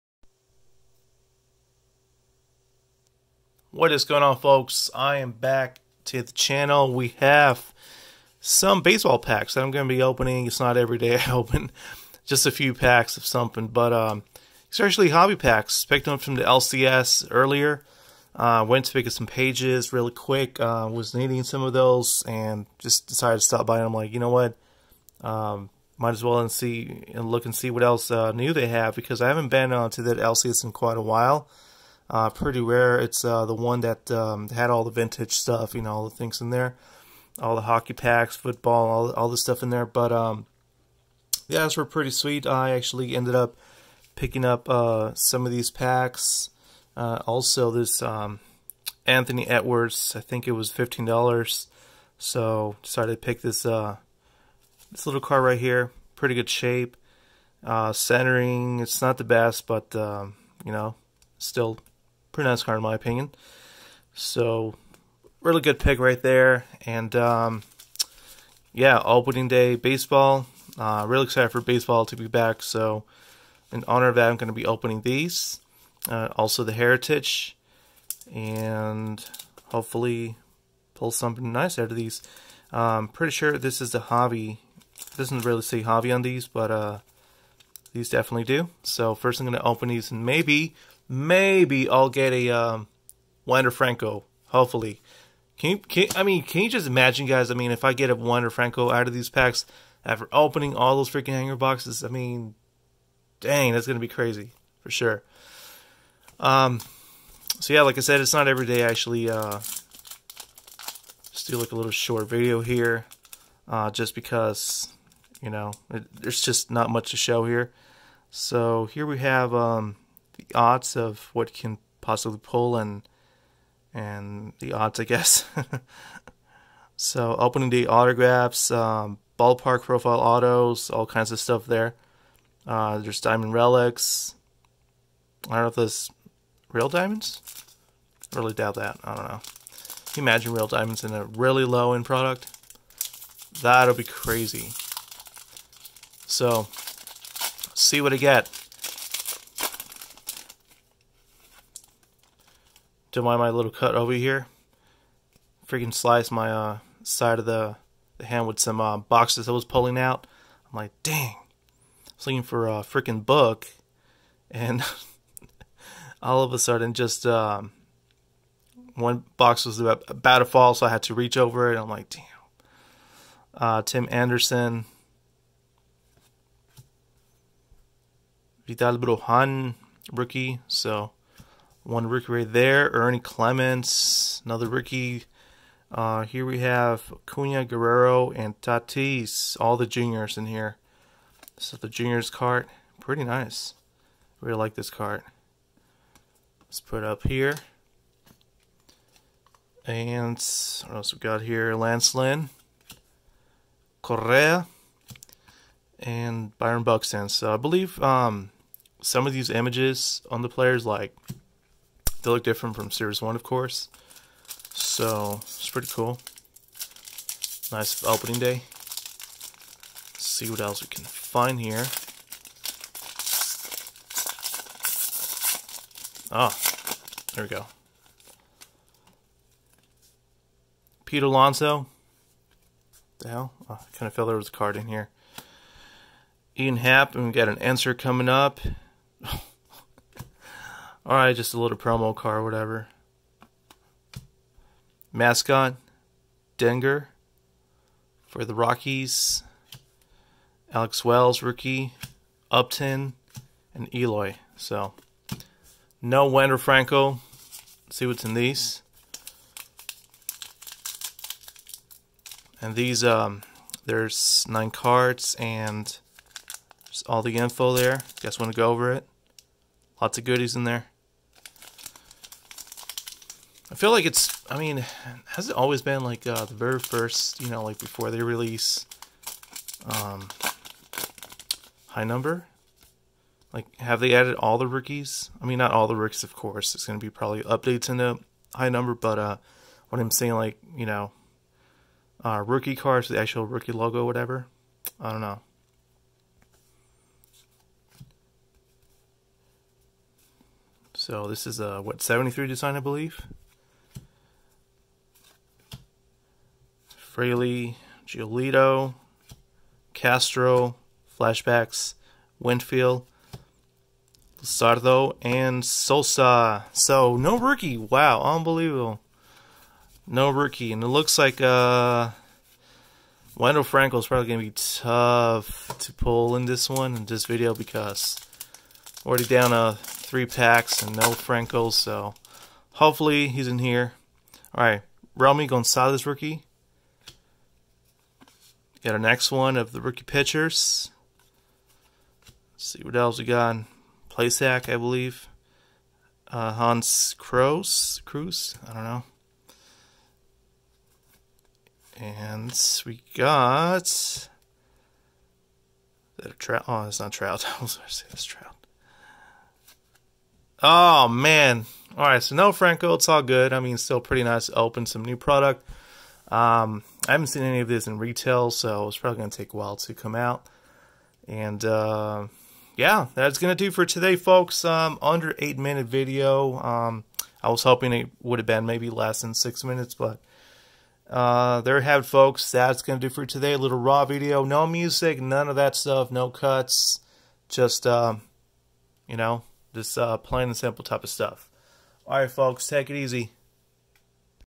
The Search For Wander Franco Continues With New Product | 2022 Topps Opening Day & Heritage Hobby!
4 hobby packs from the LCS.